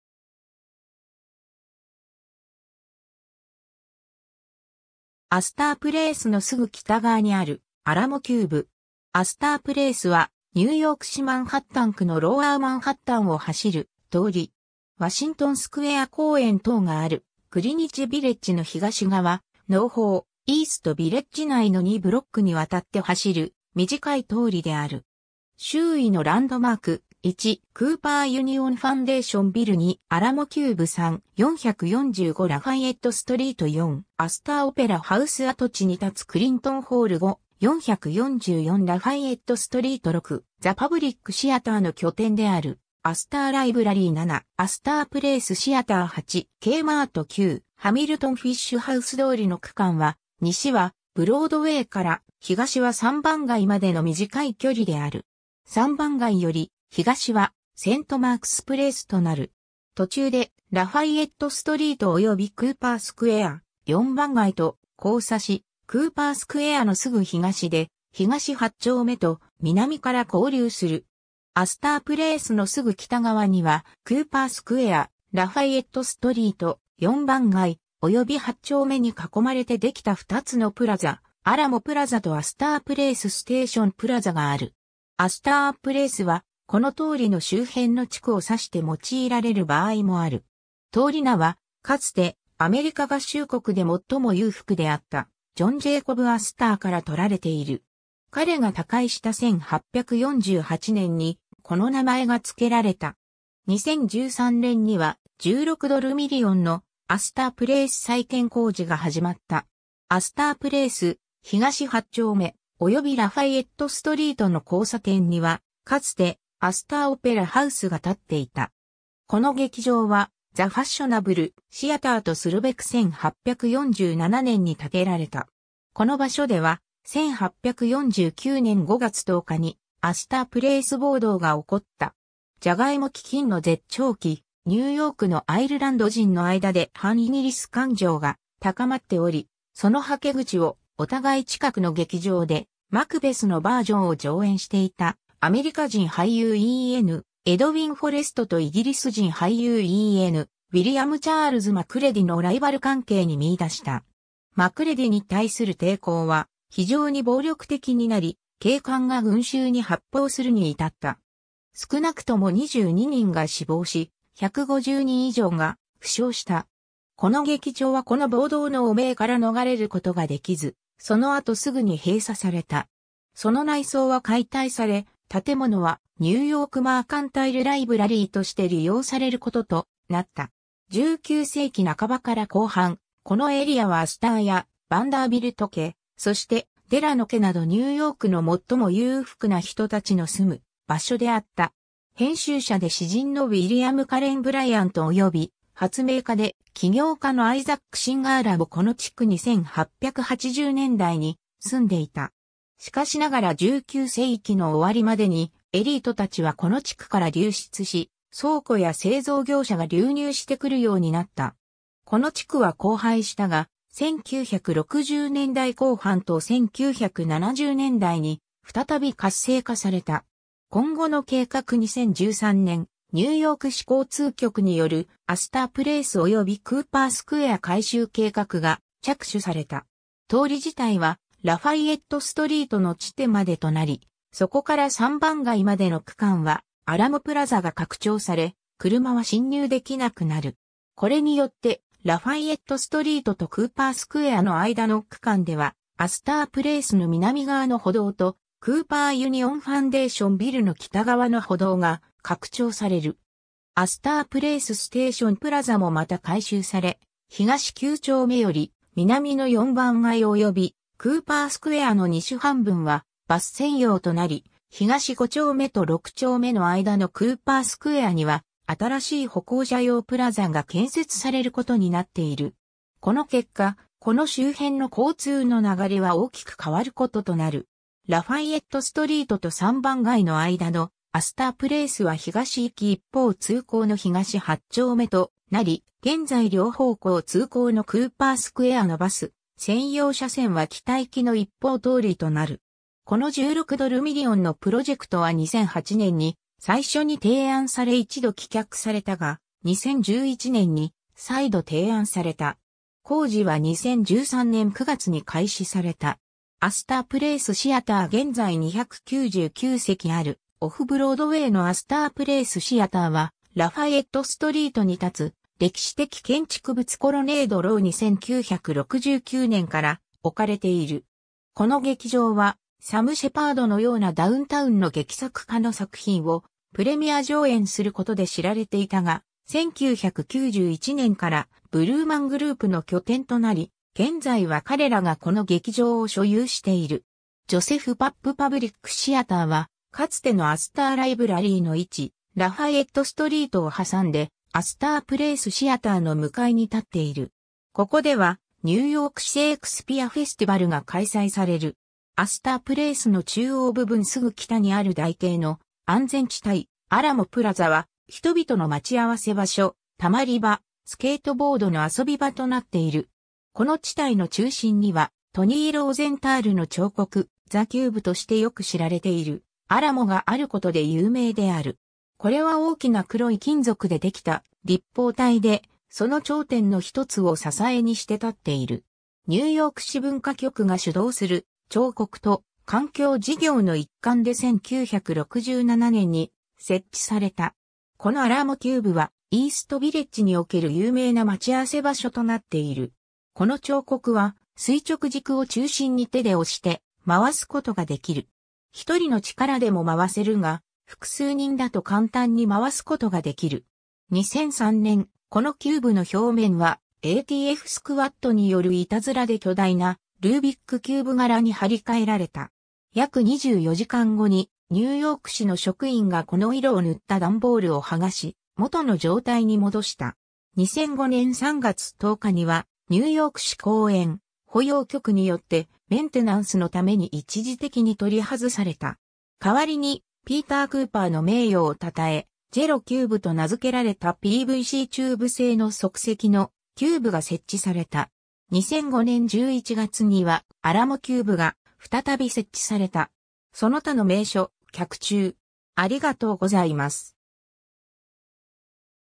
アスター・プレイス
イースト・ヴィレッジ
クーパー・ユニオン
アスター・プレイスのすぐ北側にあるアラモ・キューブ (en)。
 アスター・プレイス (Astor Place) は、ニューヨーク市マンハッタン区のロウアー・マンハッタンを走る通り。
ワシントン・スクエア公園等があるグリニッジ・ヴィレッジの東側、ノーホー / イースト・ヴィレッジ内の2ブロックにわたって走る短い通りである。
 周囲のランドマーク： 1. クーパー・ユニオン (en) ファンデーション・ビル 2. アラモ・キューブ (en) 3. 445 ラファイエット・ストリート 4. アスター・オペラ・ハウス (en) 跡地に建つクリントン・ホール (en) 5. 444 ラファイエット・ストリート 6. ザ・パブリック・シアター (en) の拠点であるアスター・ライブラリー (en) 7. アスター・プレイス・シアター (en) 8. Kマート (en) 9. ハミルトン・フィッシュ・ハウス (en) 通りの区間は、西はブロードウェイから東は3番街までの短い距離である。
3番街より東はセント・マークス・プレイスとなる。
途中でラファイエット・ストリート (en) およびクーパー・スクエア / 4番街と交差し、クーパー・スクエアのすぐ東で東8丁目と南から合流する。
 アスター・プレイスのすぐ北側には、クーパー・スクエア、ラファイエット・ストリート、4番街、および8丁目に囲まれてできた二つのプラザ（広場）、アラモ・プラザ (en) とアスター・プレイス・ステーション・プラザ (en) がある。
アスター・プレイスは、この通りの周辺の地区（ネイバーフッド）を指して用いられる場合もある。
 通り名は、かつてアメリカ合衆国でもっとも裕福であったジョン・ジェイコブ・アスターからとられている。
彼が他界した1848年に、この名前が付けられた。
 2013年には、$16ミリオンのアスター・プレイス再建工事が始まった。
 アスター・プレイス、東8丁目、およびラファイエット・ストリートの交差点には、かつてアスター・オペラ・ハウス (en) が建っていた。
この劇場は、the fashionable theater とするべく1847年に建てられた。
 この場所では、1849年5月10日にアスター・プレイス暴動 (en) が起こった。
ジャガイモ飢饉の絶頂期、ニューヨークのアイルランド人の間で反イギリス感情が高まっており、そのはけ口をお互い近くの劇場でマクベスのバージョンを上演していたアメリカ人俳優en:Edwin Forrestとイギリス人俳優en:William Charles Macreadyのライバル関係に見出した。
Macreadyに対する抵抗は非常に暴力的になり、警官が群衆に発砲するに至った。
少なくとも22人が死亡し、150人以上が負傷した。
この劇場はこの暴動の汚名から逃れることができず、その後すぐに閉鎖された。
その内装は解体され、建物はニューヨーク・マーカンタイル・ライブラリー (en) として利用されることとなった。
 19世紀半ばから後半、このエリアはアスター家、ヴァンダービルト家、そしてデラノ家などニューヨークの最も裕福な人たちの住む場所であった。
編集者で詩人のウィリアム・カレン・ブライアントおよび発明家で企業家のアイザック・シンガーらもこの地区に1880年代に住んでいた。
しかしながら19世紀の終わりまでに、エリートたちはこの地区から流出し、倉庫や製造業者が流入してくるようになった。
この地区は荒廃したが、1960年代後半と1970年代に再び活性化された。
 今後の計画2013年、ニューヨーク市交通局 (en) による"アスター・プレイスおよびクーパー・スクエア改修" (Reconstruction of Astor Place and Cooper Square) 計画が着手された。
通り自体はラファイエット・ストリートの地点までとなり、そこから3番街までの区間はアラモ・プラザ (en) が拡張され、車は侵入できなくなる。
これによって、ラファイエット・ストリートとクーパー・スクエアの間の区間ではアスター・プレイスの南側の歩道とクーパー・ユニオン・ファンデーション・ビルの北側の歩道が拡張される。
アスター・プレイス・ステーション・プラザ (en) もまた改修され、東9丁目より南の4番街およびクーパー・スクエアの西半分はバス専用となり、東5丁目と6丁目の間のクーパー・ス...